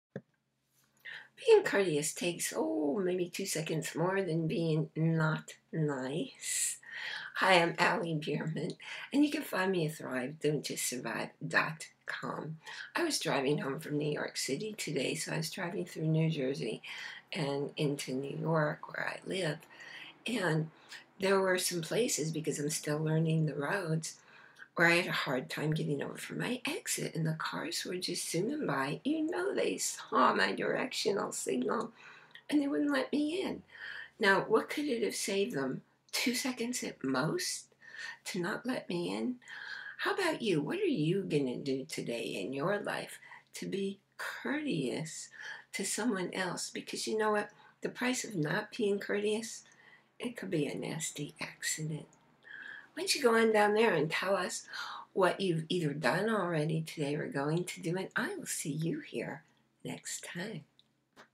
Courtesy, Honor and Respect TAT Ali Bierman
Being courteous only take a moment and can prevent a traumatic event, a misunderstanding and a host of other not very good things in life.